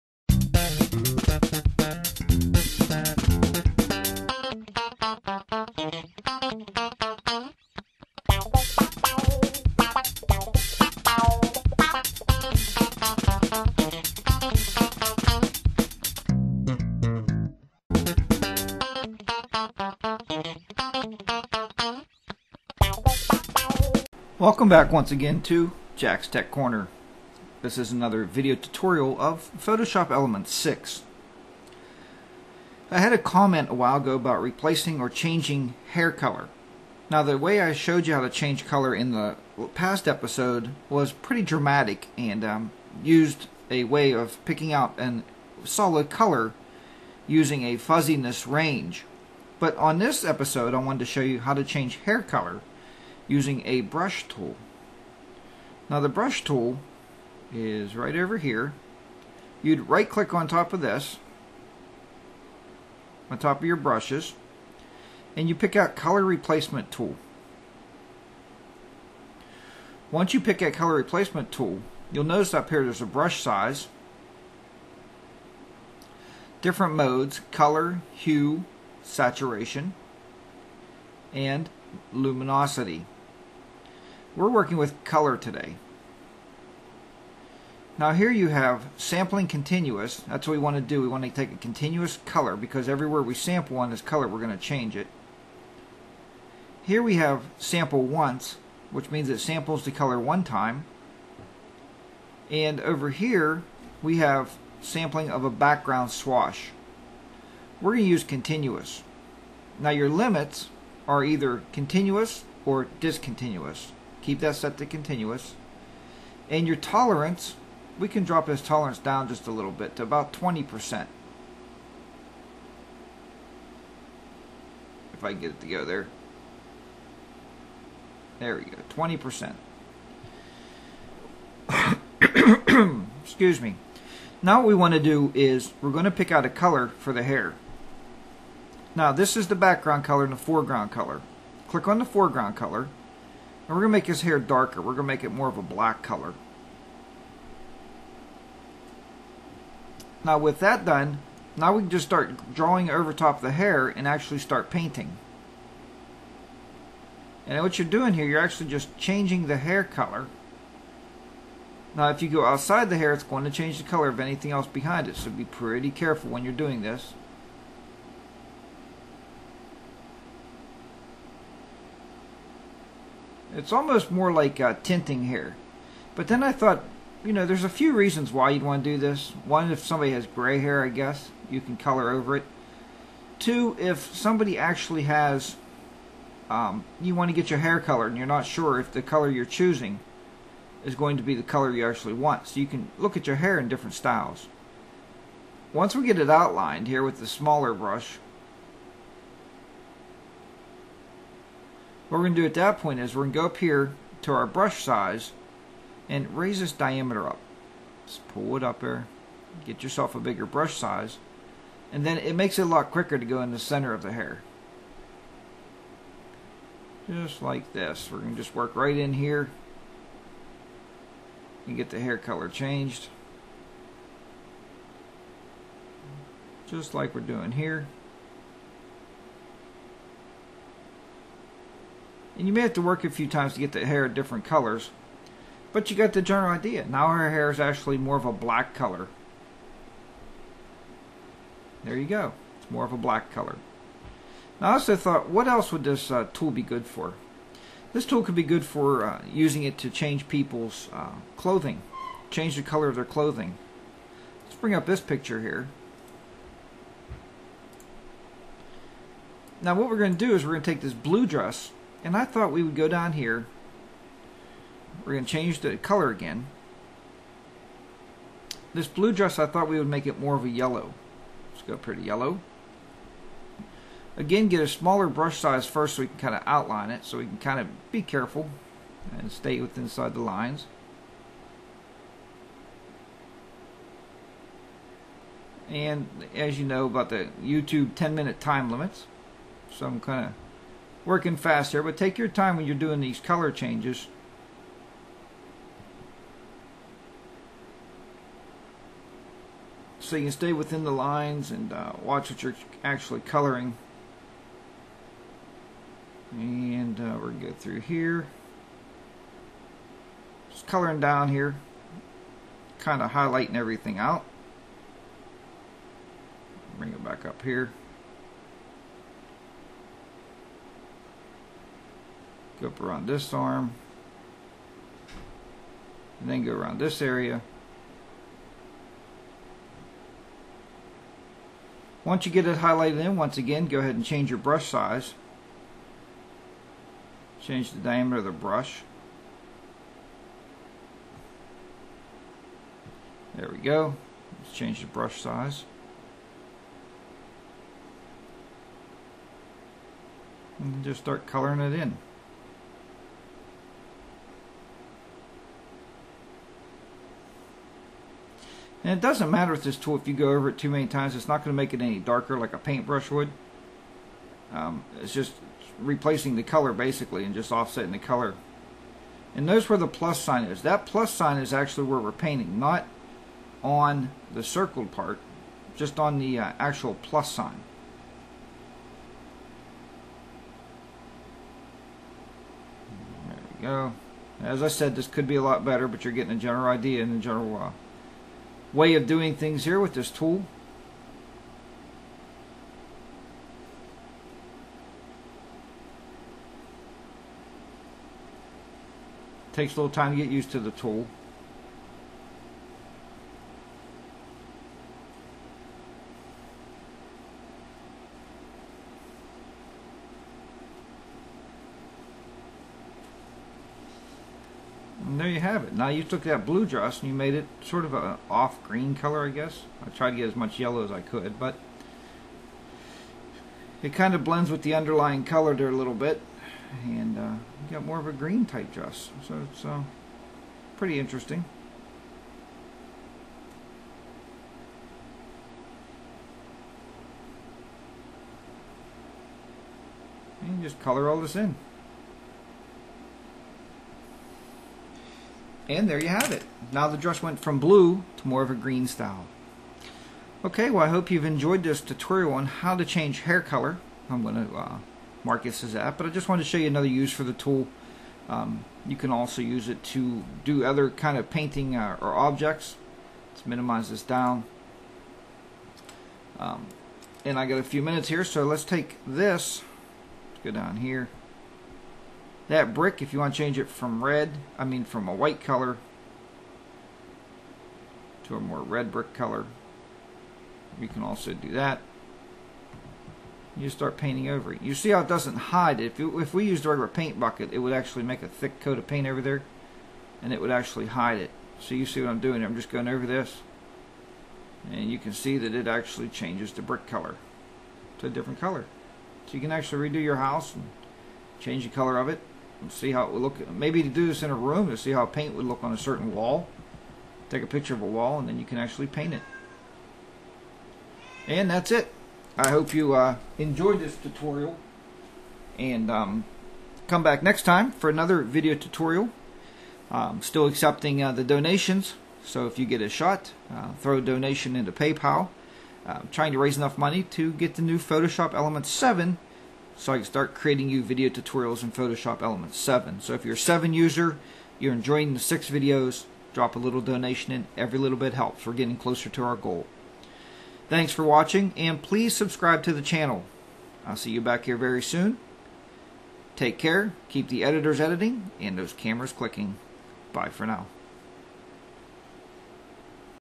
Changing Hair Color Photoshop Elements 6
In this video I will show you how to use the color replacement tool. This is a great tool for changing hair color and more.